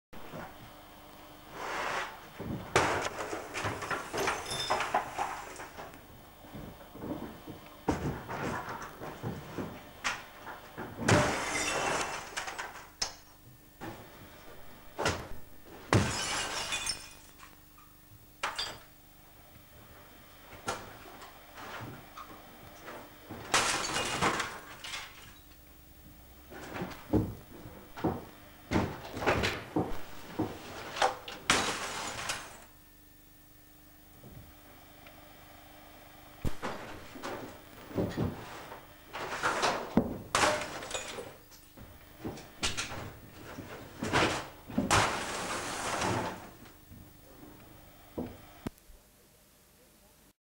Dropping some TVs down the stairs
Dropping some broken TVs from the loft down on a concrete floor. It's a 4-5 meter drop.